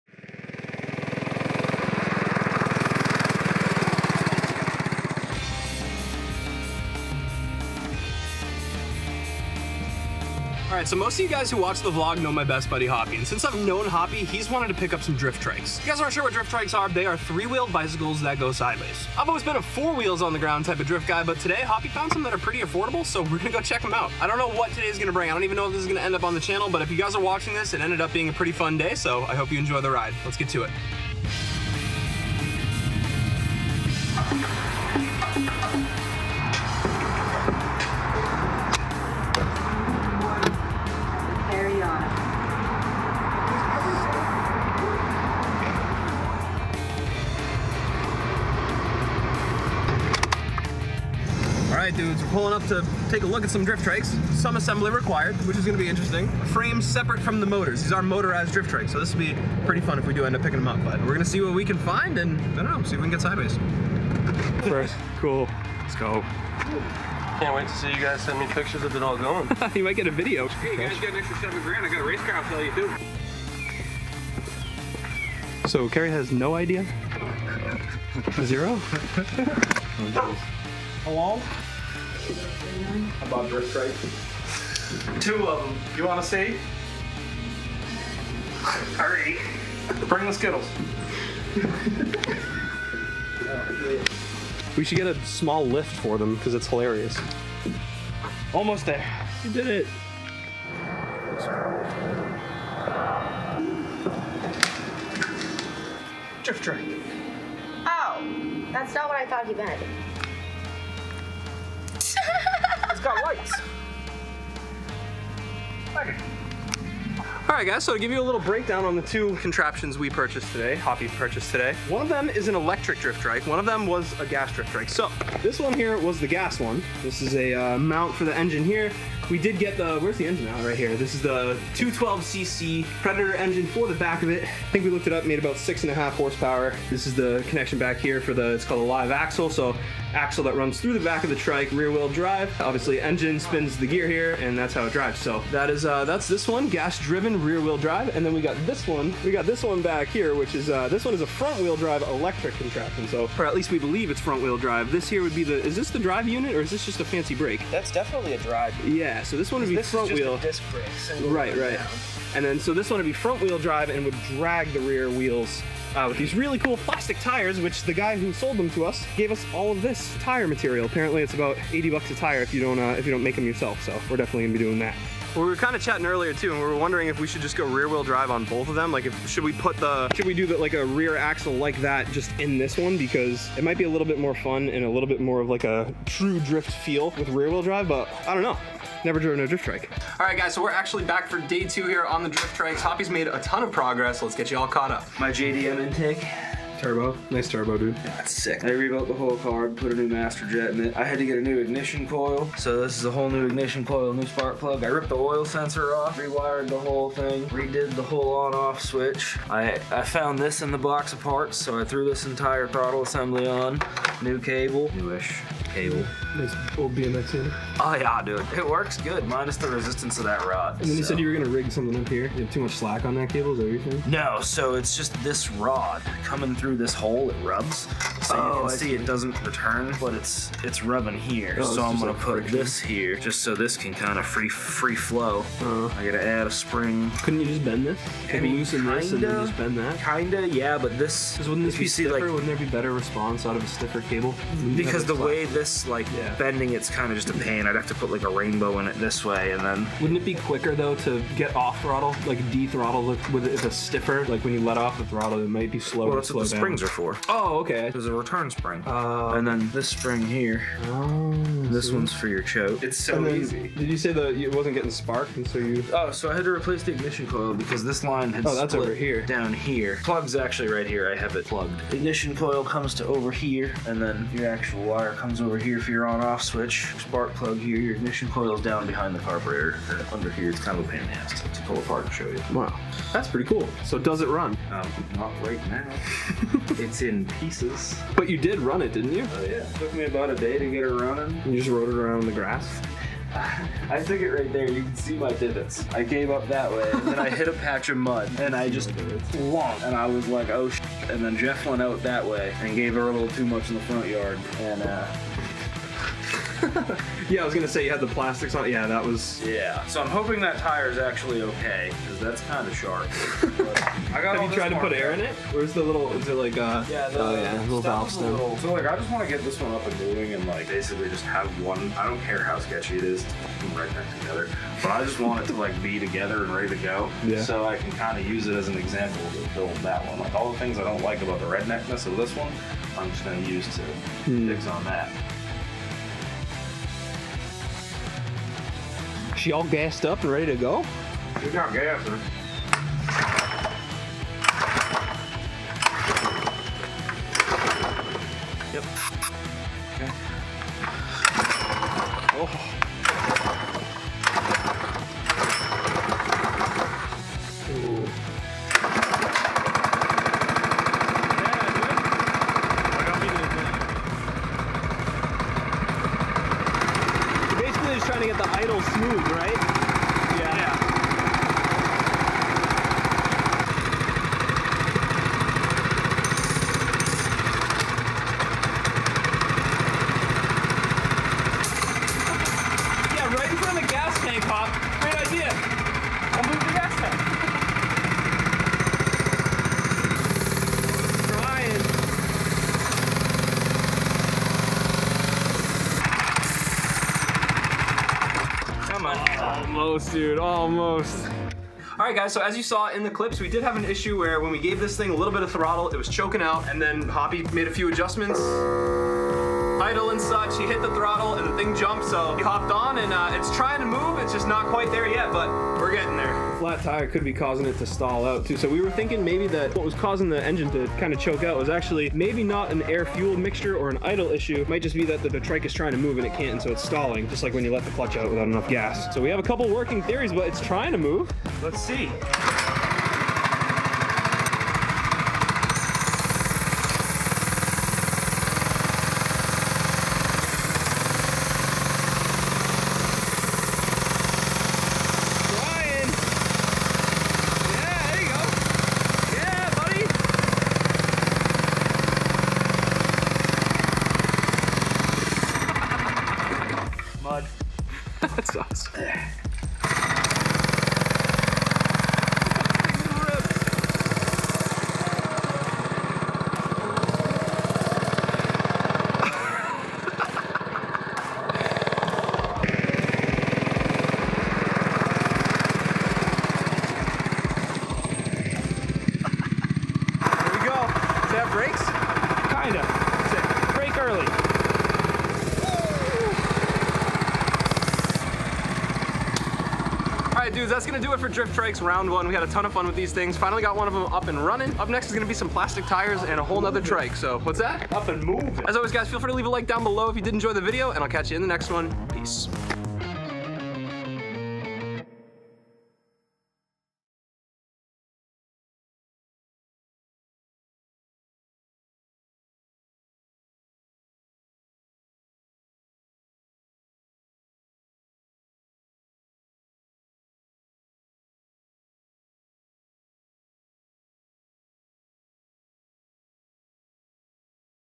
Predator Engine Drift Trike Restoration - We Got a RUNNER!?
Restoring & rebuilding a set of drift trikes!

M O R E V L O G S !